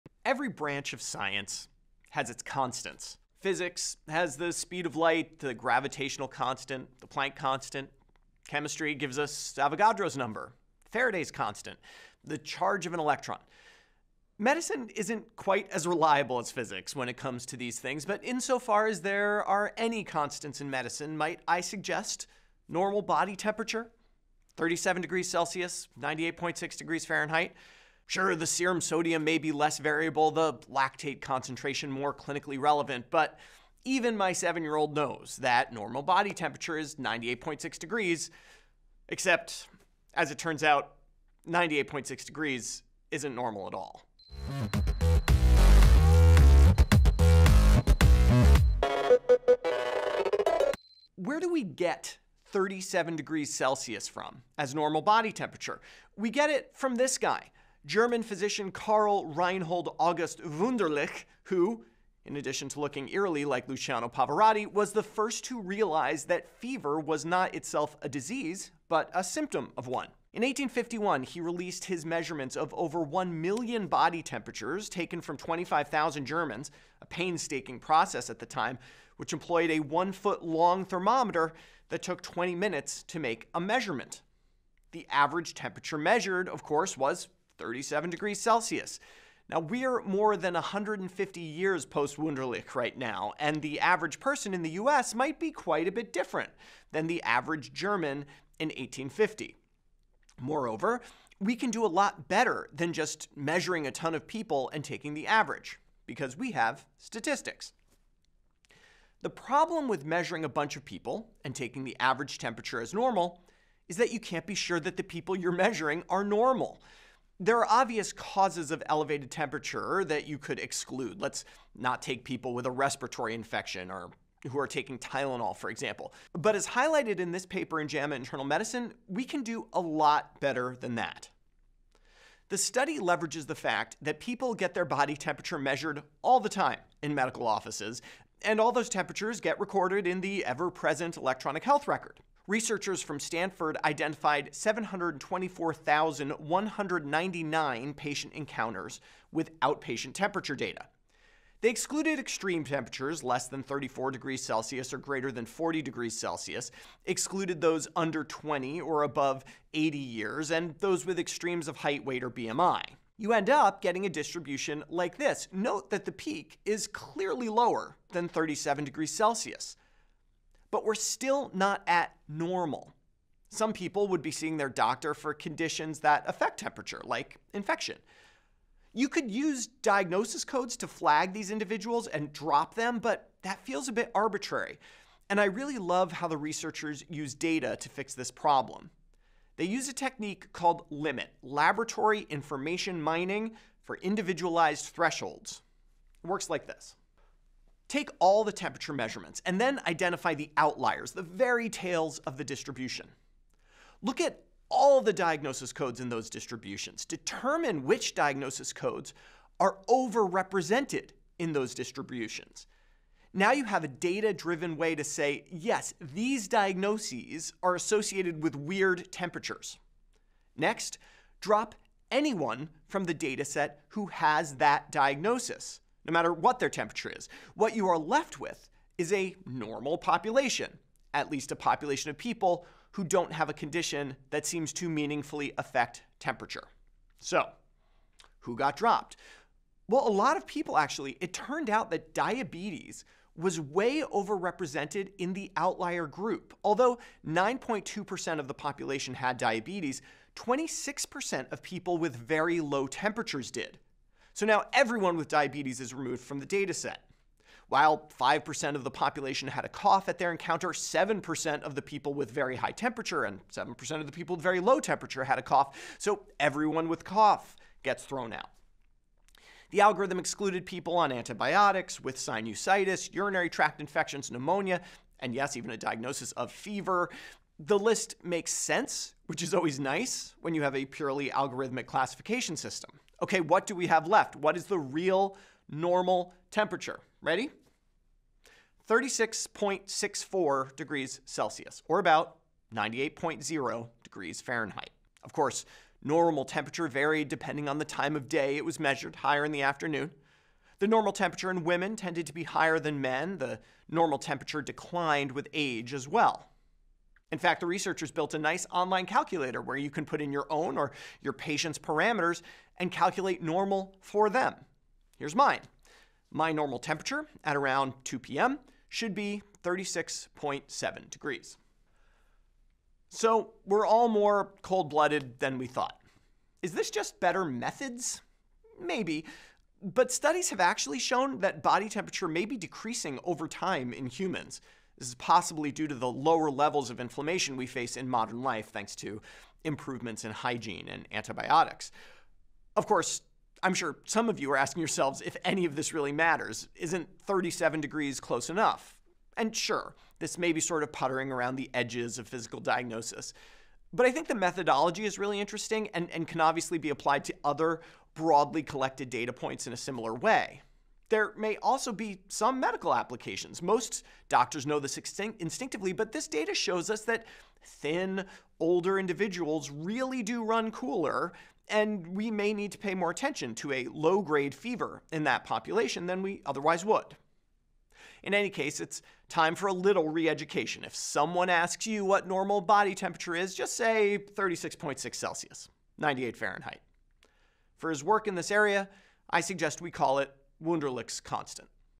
Normal Body Temperature is Actually 98.0 Degrees (F)
Every branch of science has its constants. Physics has the speed of light, the gravitational constant, the Planck constant. Chemistry gives us Avogadro’s number, Faraday’s constant, the charge of an electron. Medicine isn’t quite as reliable as physics when it comes to these things, but insofar as there are any constants in Medicine, might I suggest normal body temperature. Thirty-seven degrees Celsius, 98.6 degrees Fahrenheit.

Sure, the serum sodium may be less variable, the lactate concentration more clinically relevant, but even my seven-year-old knows that normal body temperature is 98.6 degrees.

Except, as it turns out. 98.6 degrees isn’t normal at all.